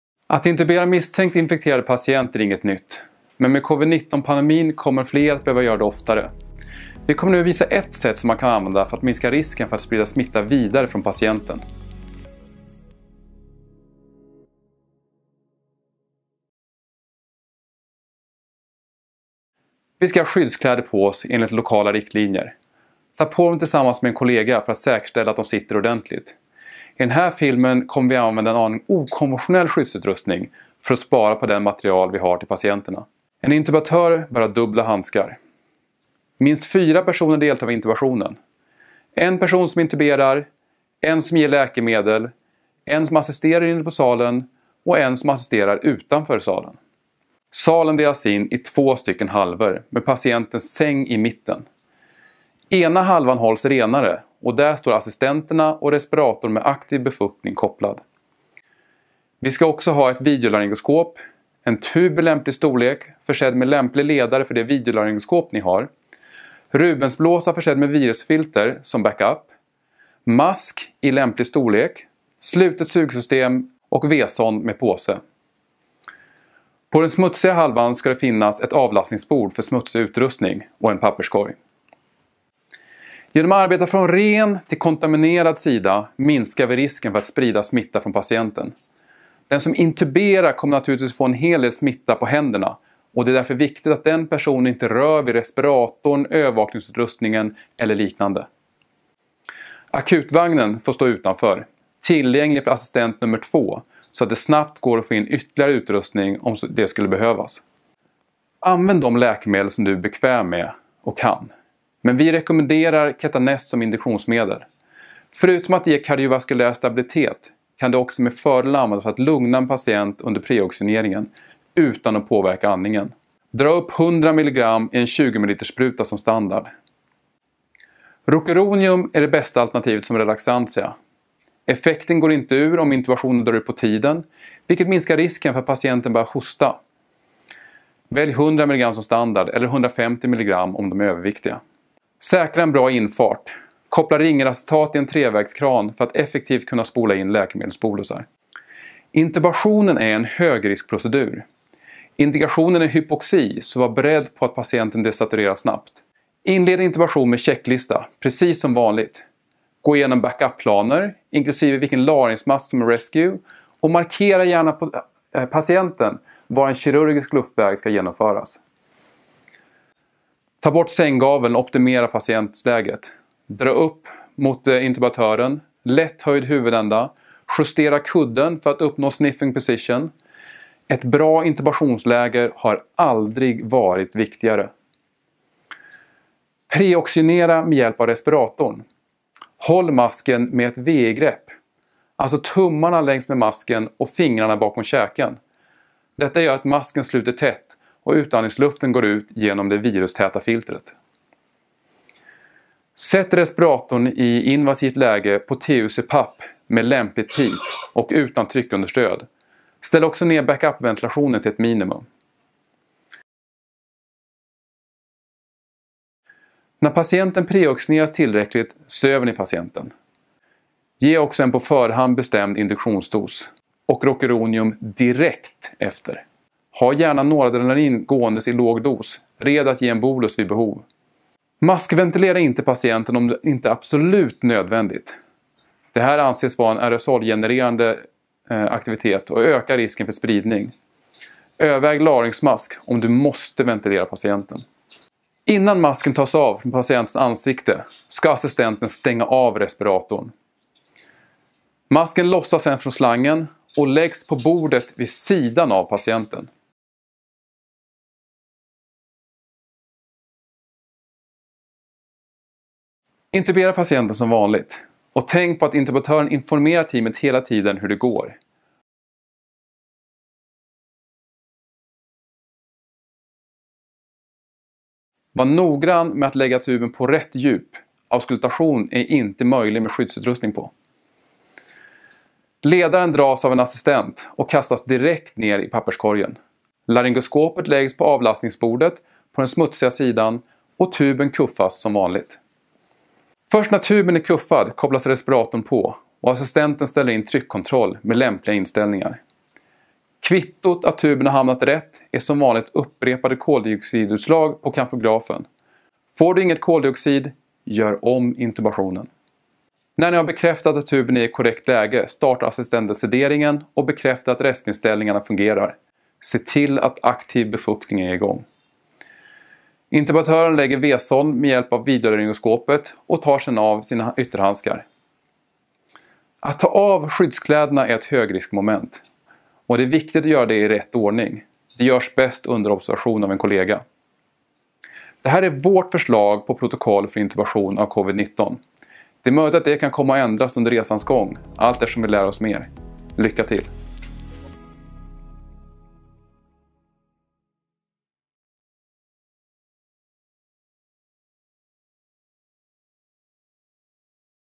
COVID-19 IS AIRBORNE NOW OR NOT
Dr Willhem Wallquist går igenom vårt förslag på protokoll för intubation av misstänkta COVID-19 patienter. Detta vid Skånes Universitetssjukhus i Lund, och kan komma att ändras.

Dr Wilhelm Wallquist guides us through our suggested protocol for intubation in patients with suspected or confirmed COVID-19. Filmed at Skåne University Hospital, Lund, and subject to change.